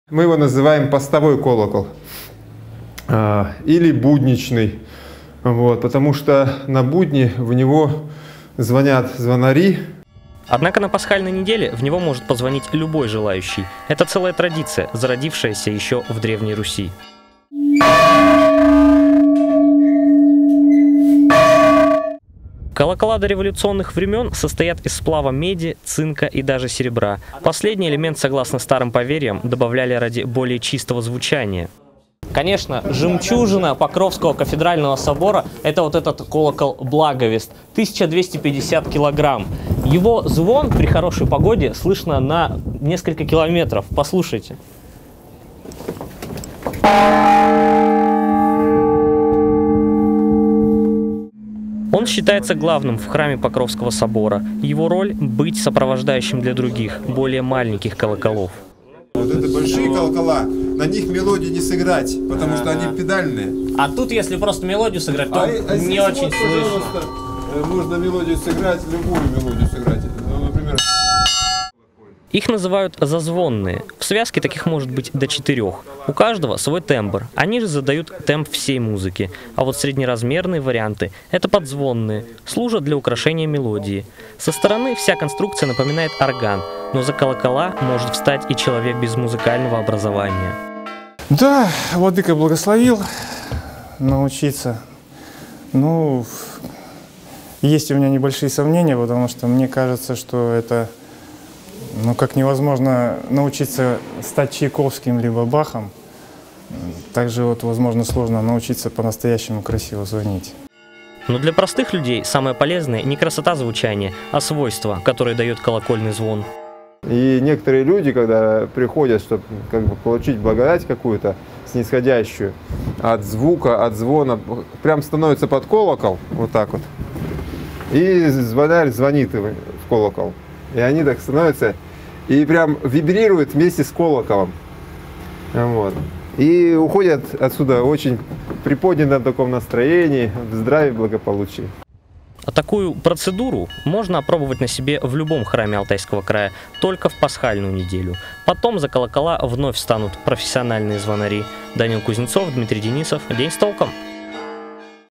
В Барнауле на Светлую седмицу в колокол может ударить любой желающий
На этой неделе во всех храмах Алтайского края колокольный звон будет раздаваться не по расписанию. Корреспондент "ТОЛКа" постигал азы колокольного звона и узнавал, зачем нужно звонить в колокола.